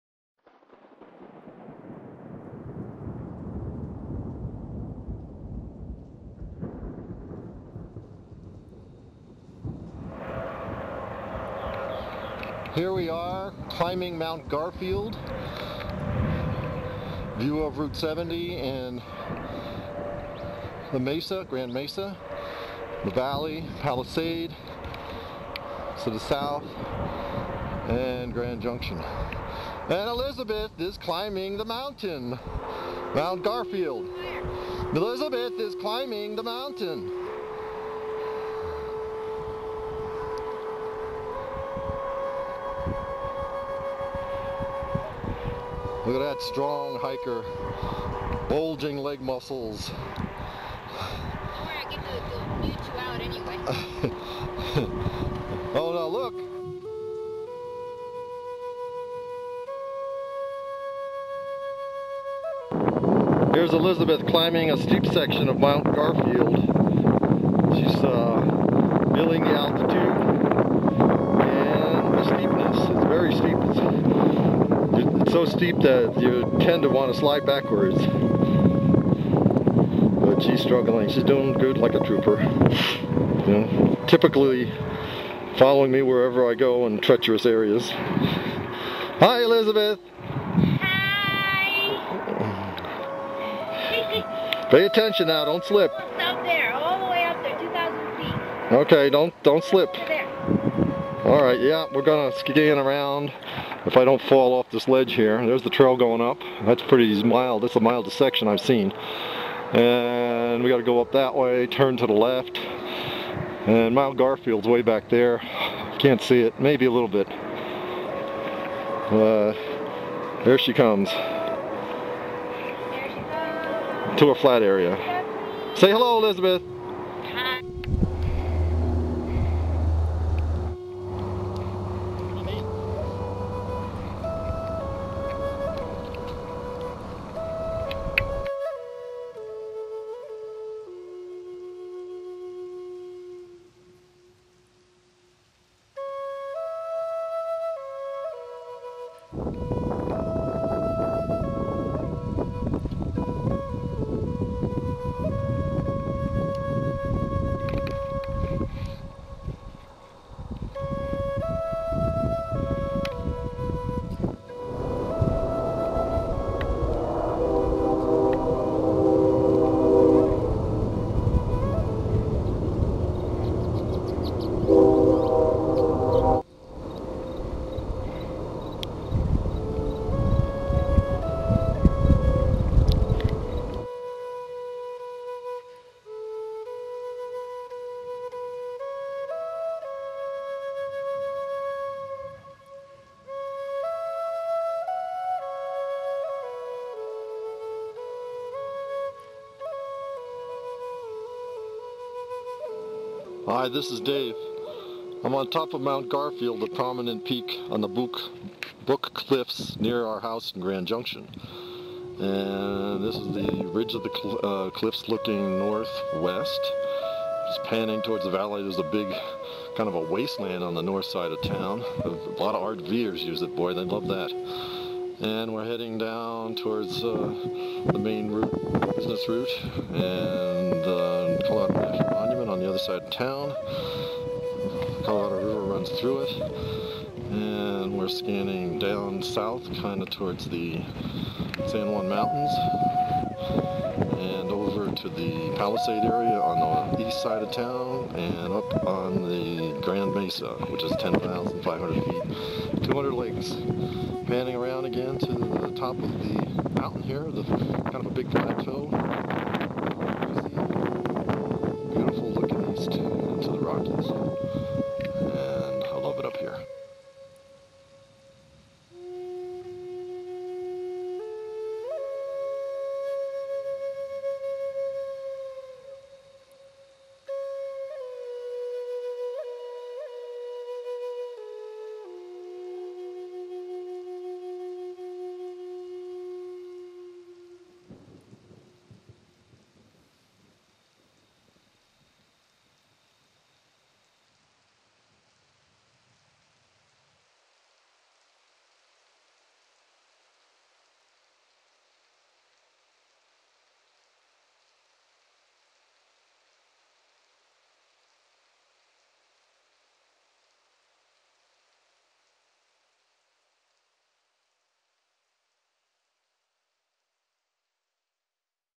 Hiking Mt Garfield - Grand Junction CO
Video/pics slide show of us hiking Mt Garfield.  We started from Gearhart Mine trail adding 1 extra mile - some say is less steeper - (don't agree)but we did come down Mt Garfield trail.